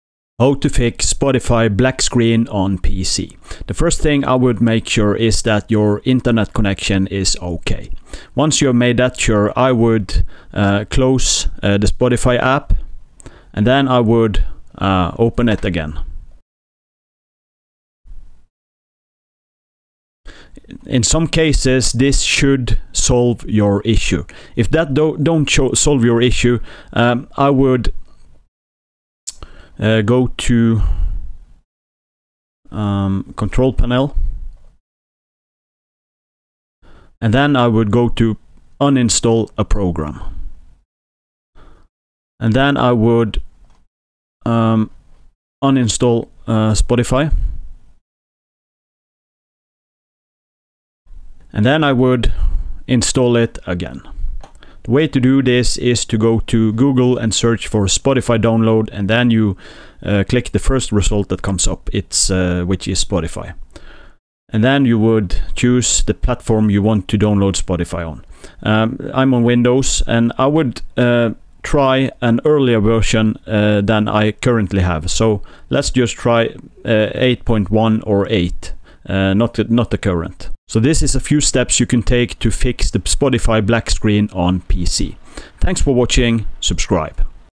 How to fix Spotify black screen on pc- FULL GUIDE- (2025)
In this video I show you how to fix Spotify black screen on pc.